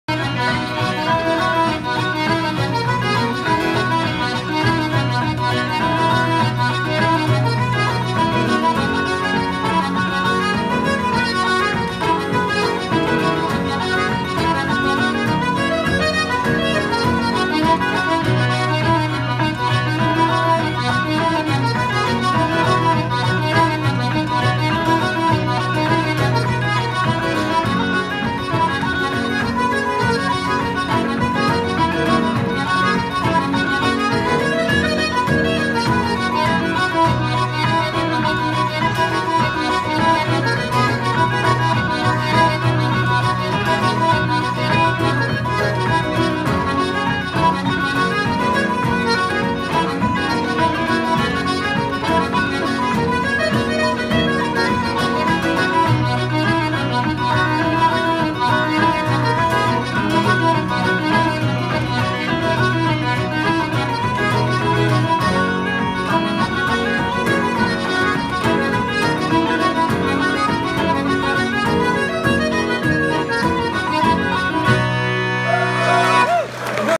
Garadice
Garadice (Eleanor Shanley, Dave Sheridan, Padraig McGovern, John McCartin) Playing at the Ed Reavy Festival in The Cavan Crystal Hotel 2017